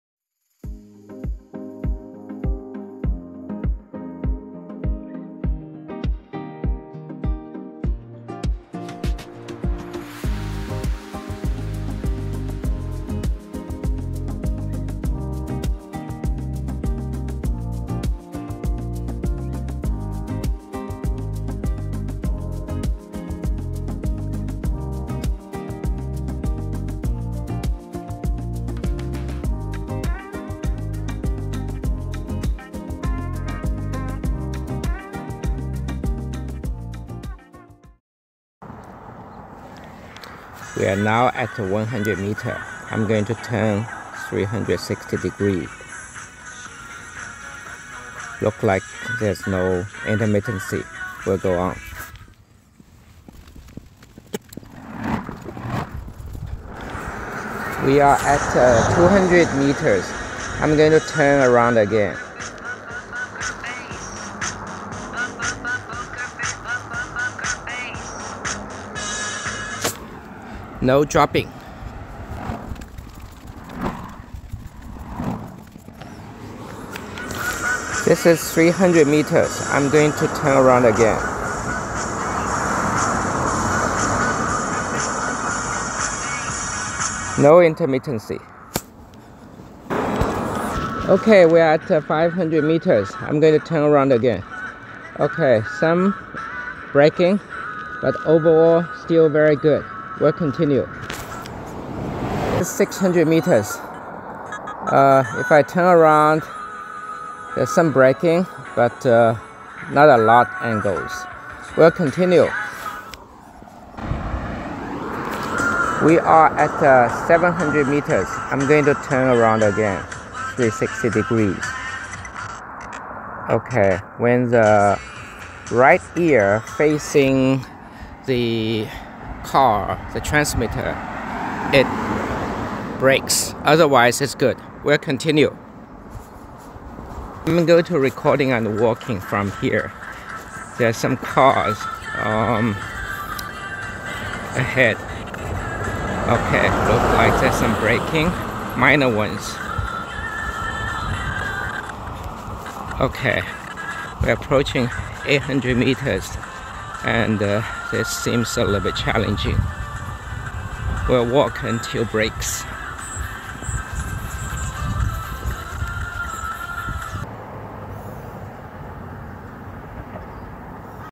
Homespot Auracast Broadcaster BA210 800m Range Extension with QCC3086 SoC and BHWA251 PA
For related information please see post in LinkedIn Group "RF for IoT"